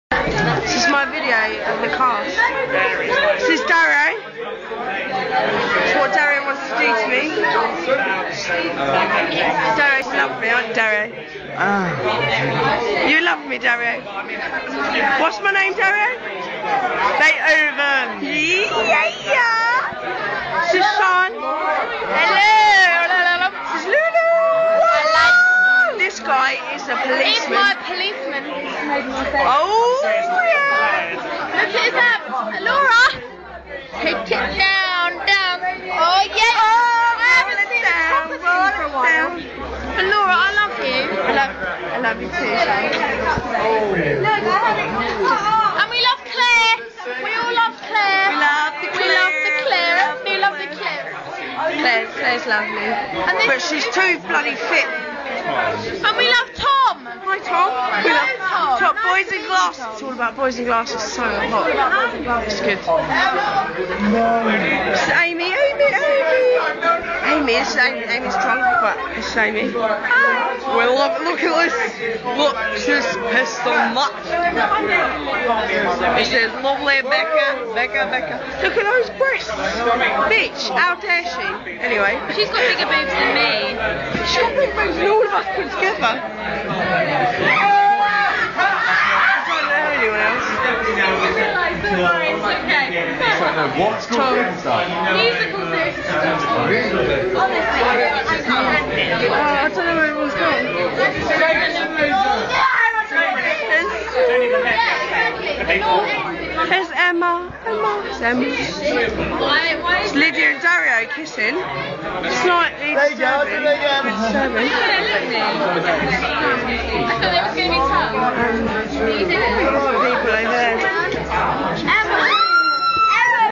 Fidler Cast at the bar
This video I also drunkanly made at the bar after our final fidler show.